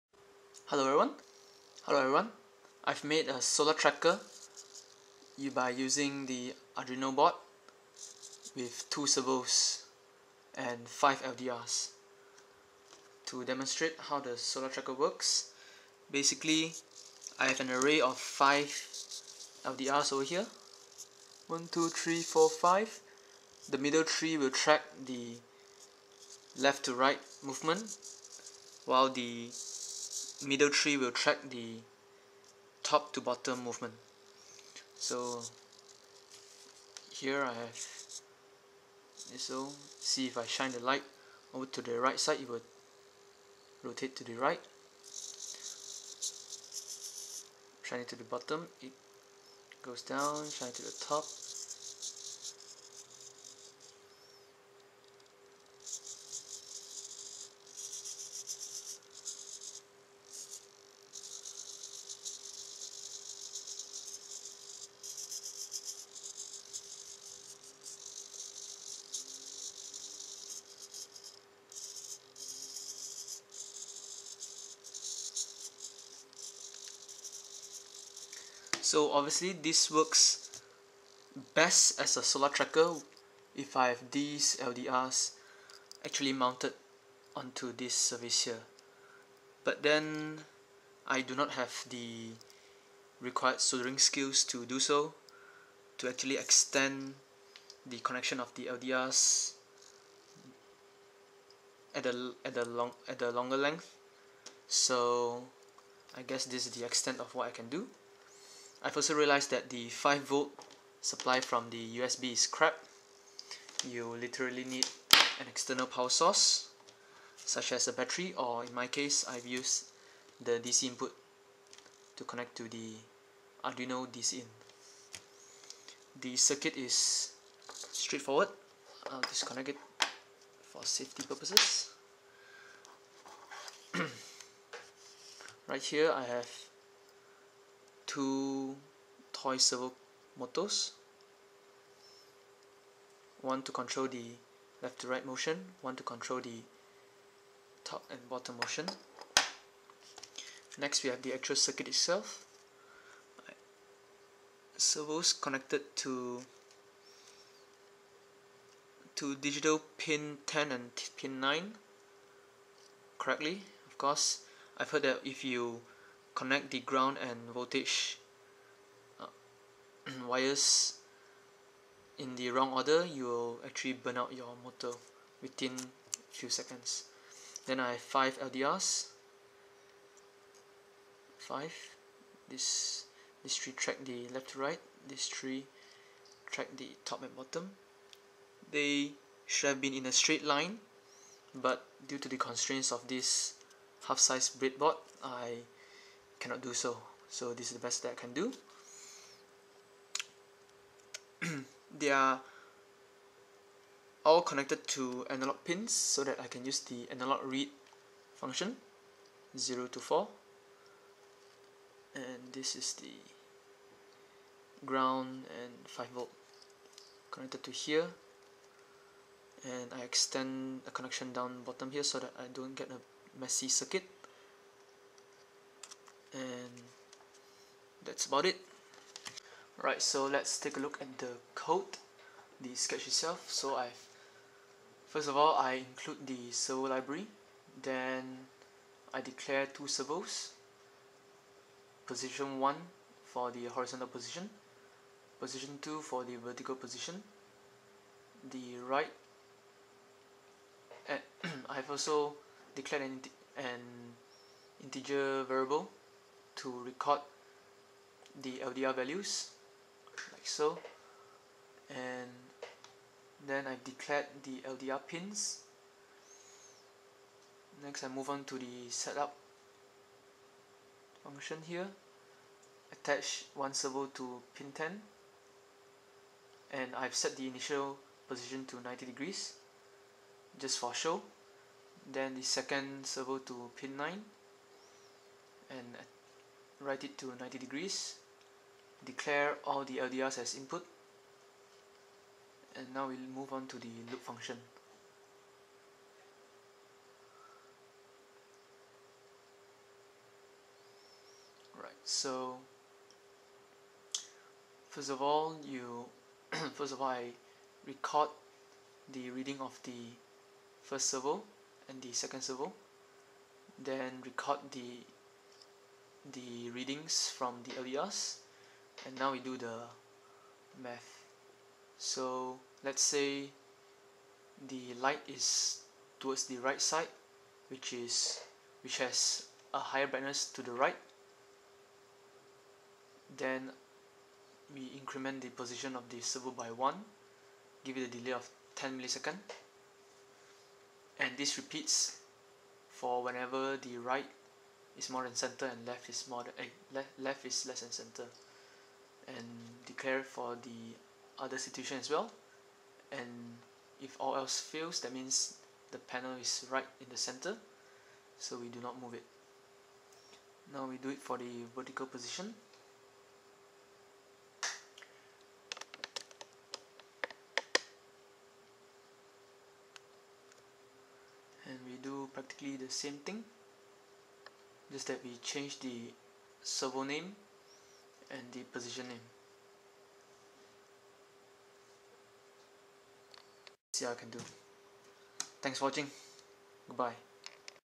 Arduino Solar Tracker (simple code)
A simple solar tracker I made using LDRs as sensors. A bulk of the codes are just if- statements.